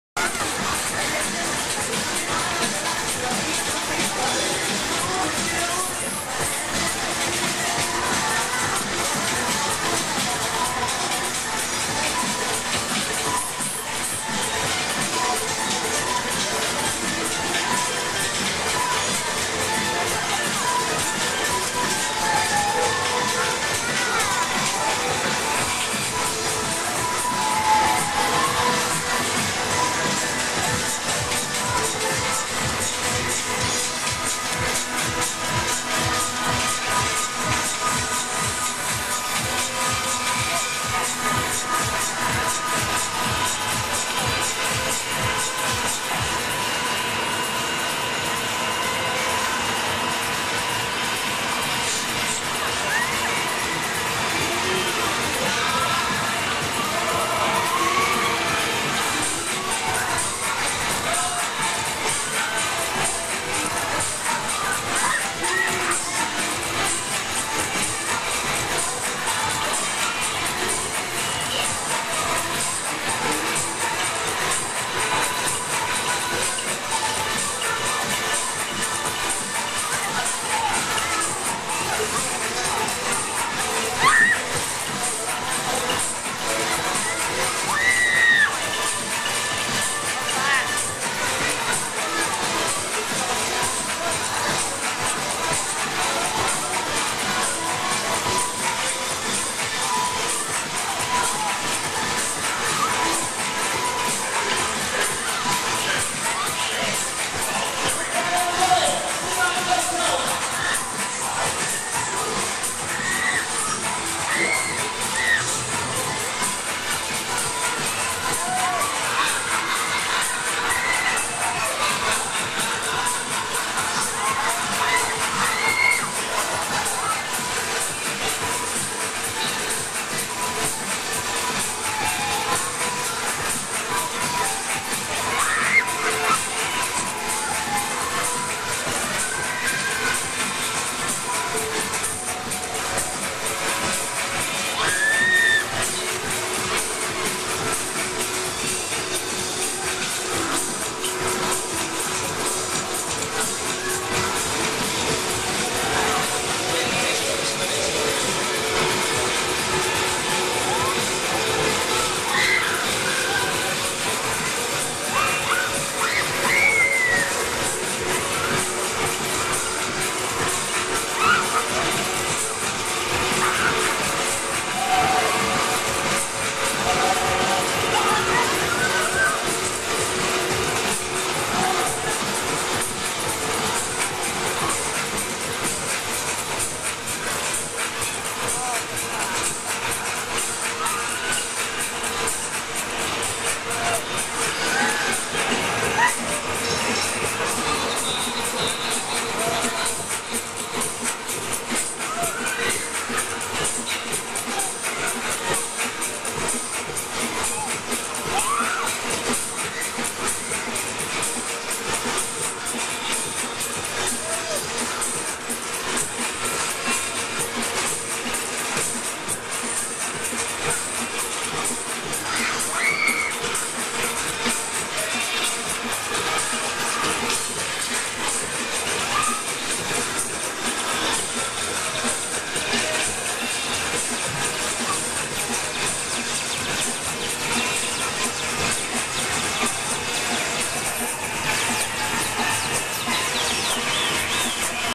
Extreme - John Parrish - Offride
Offride  of John Parrish's Extreme. Filmed at Embankment, Peterborough, Cambridgeshire, UK.